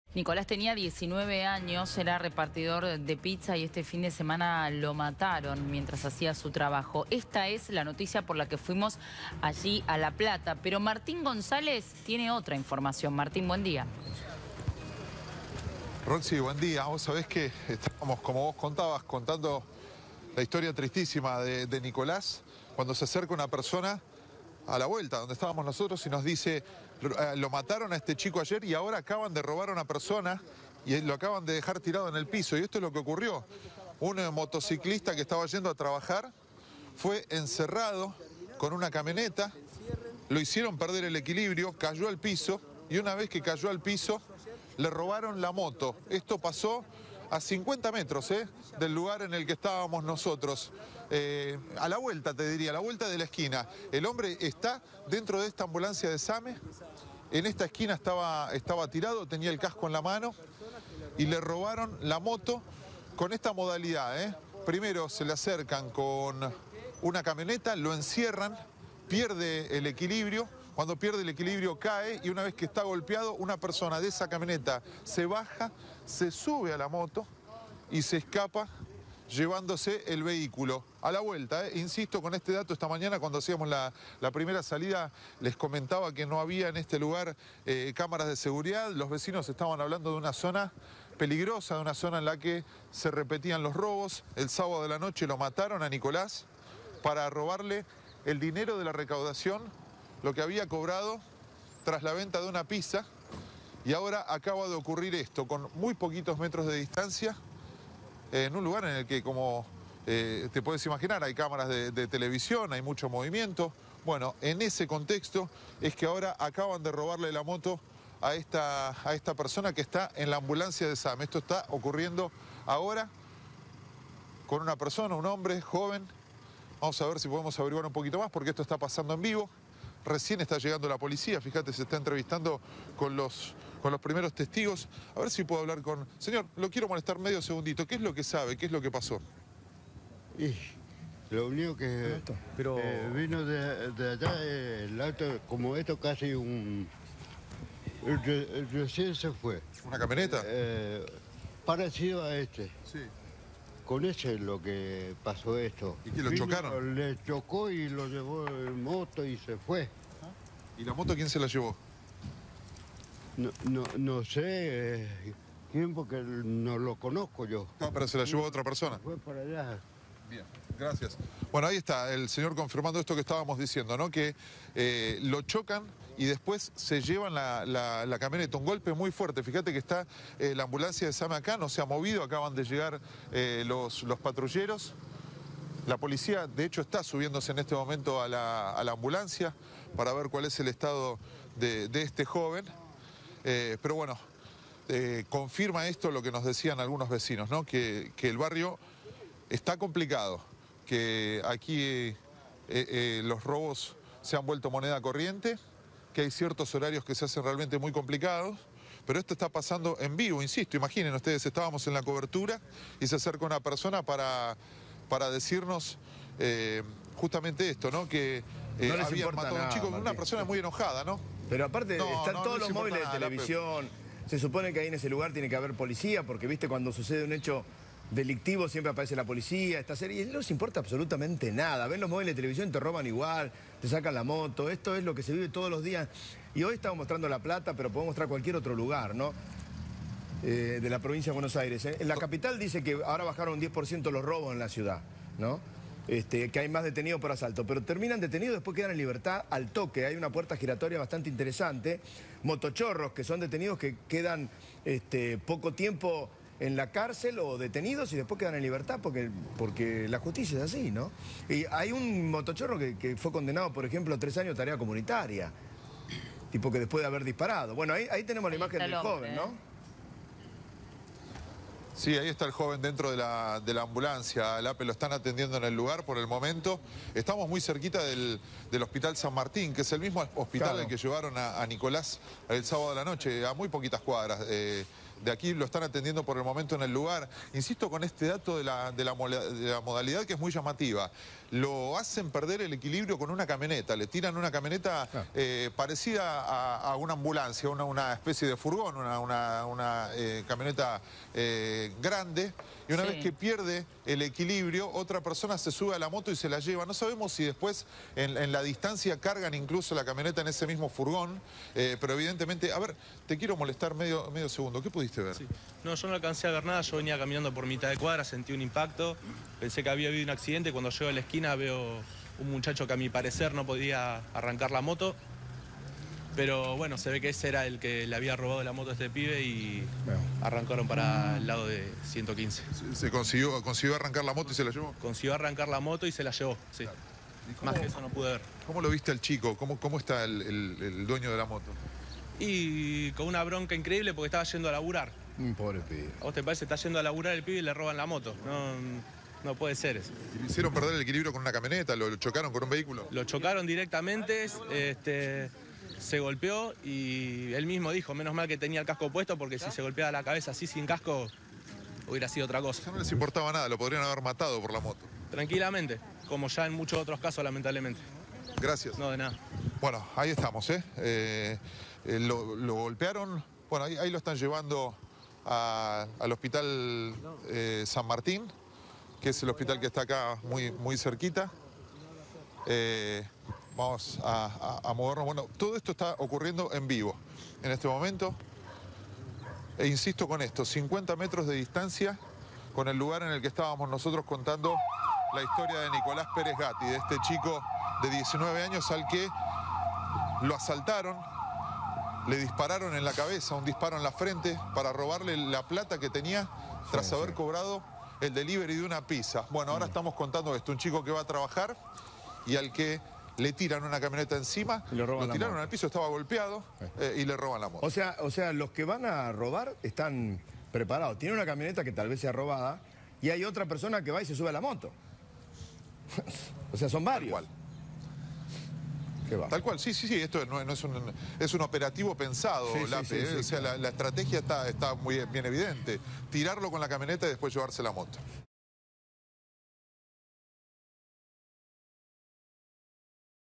A metros del lugar en el que mataron al repartidor de pizza, un robo
A metros del lugar en el que mataron a Nicolás, el repartidor de pizza asesinado en La Plata, un robo. Atacaron a un motociclista para robarle la moto: lo encerraron con una camioneta, perdió el equilibrio y cuando estaba en el suelo le robaron la moto. Los vecinos denuncian que es una zona liberada.